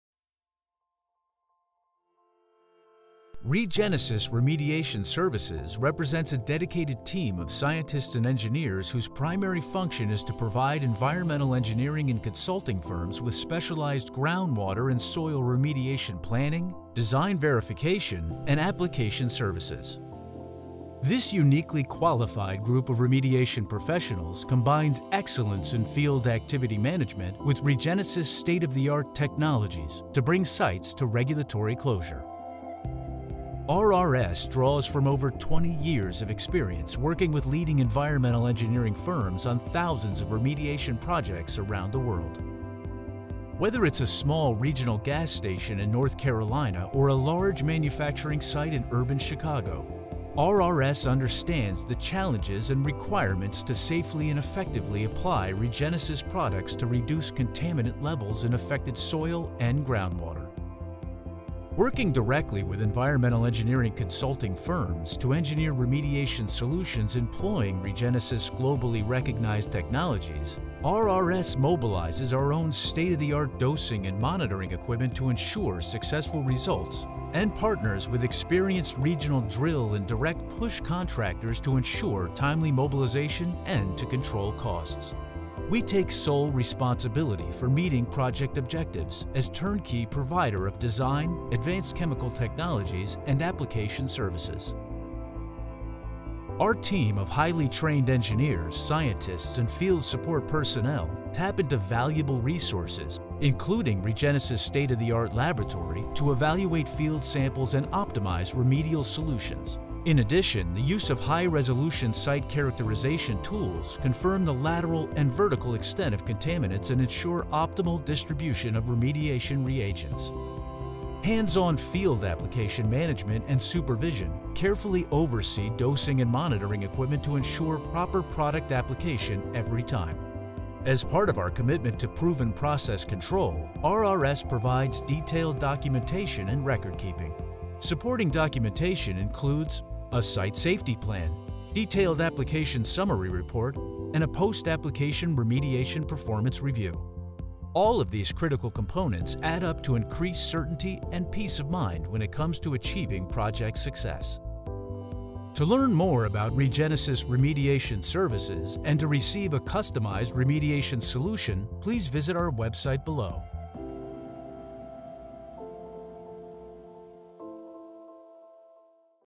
REGENESIS® Remediation Services (RRS)
REGENESIS® Remediation Services (RRS) is a dedicated team of scientists and engineers whose primary job is to provide environmental engineering and consulting firms with specialized groundwater and soil remediation planning, design, verification and on-site application services.

This uniquely qualified group of remediation professionals combine excellence in field activity management and REGENESIS’ world class-leading technologies to achieve site closure on-time and on-budget. RRS draws from 20+ years of hands-on experience working with hundreds of leading environmental engineering firms on thousands of in situ groundwater and soil remediation projects around the world.